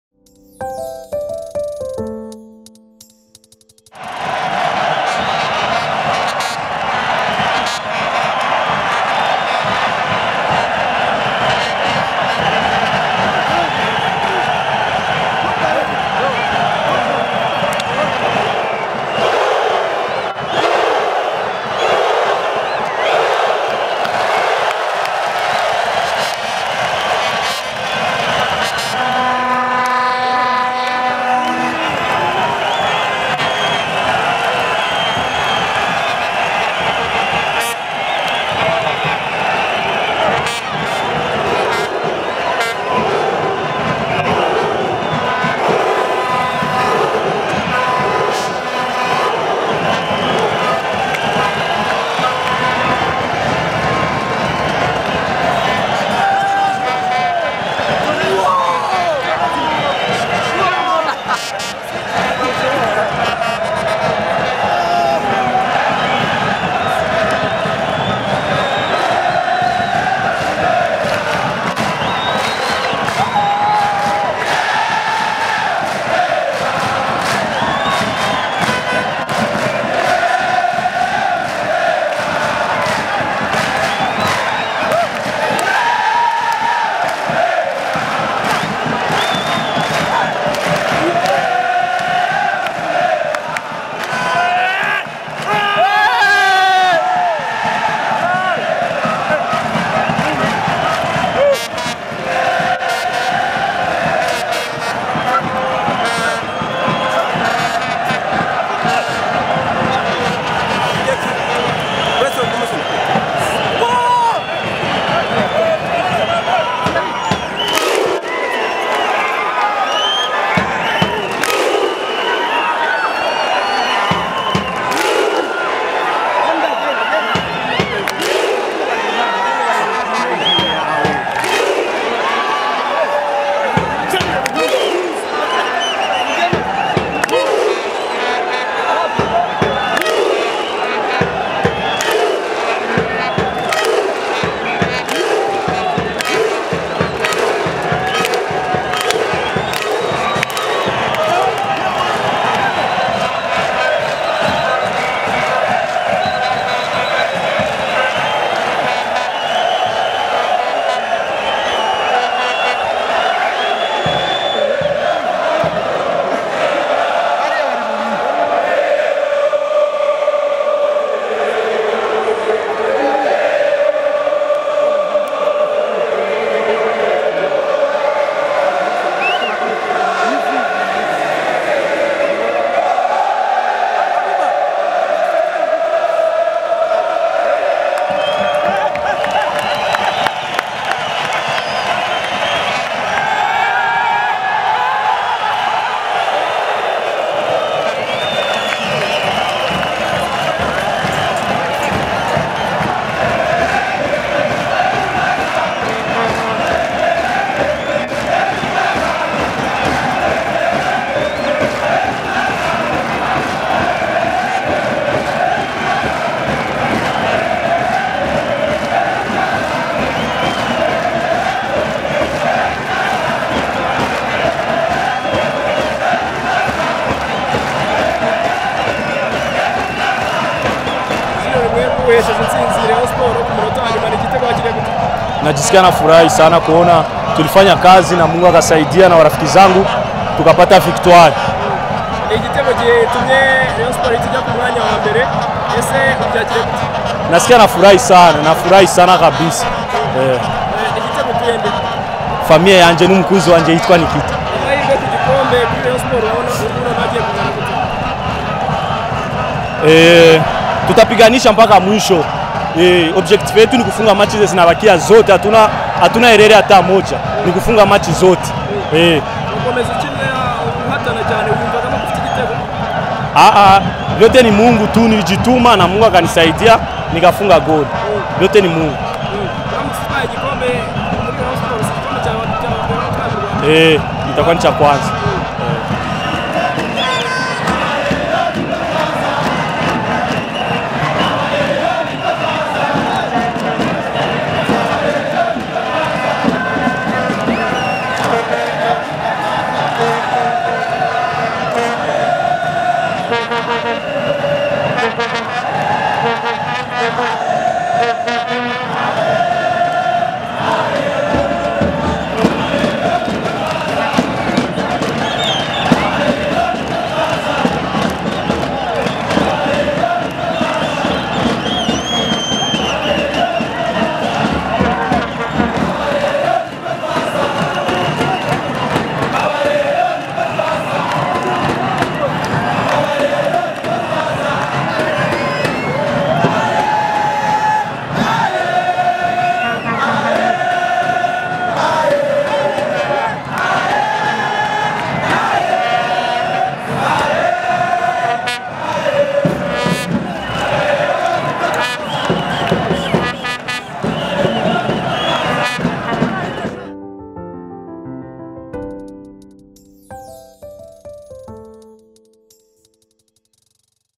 Police FC 0-1 Rayon Sports:Ibihumbi by'abafana ba Rayon Sports bishimiye gufata umwanya wa mbere
Jules Ulimwengu yahesheje Rayon Sports amanota 3 imbere ya Police FC. Gutsinda uyu mukino bihesheje Rayon Sports gufata umwanya wa mbere n’amanota 60, irusha inota rimwe APR FC ya kabiri n’amanota 59 mu gihe hasigaye imikino ine gusa ngo shampiyona irangire.

Ibihumbi by'abafana bishimiye cyane iyi ntsinzi yabonetse ku munota wa 86